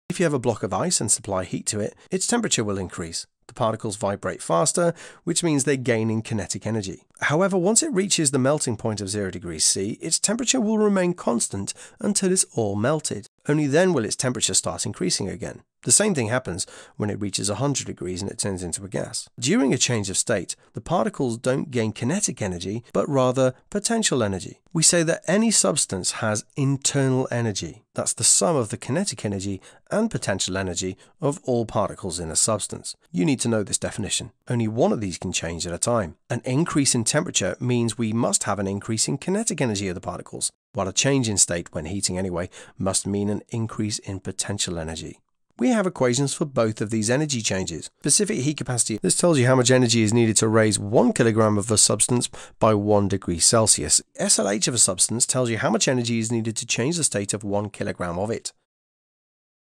INTERNAL ENERGY - Specific Heat Capacity & Latent Heat - Physics Science Revision (GCSE)
I don't charge anyone to watch my videos, so please Super Thanks/donate if you can to keep me going!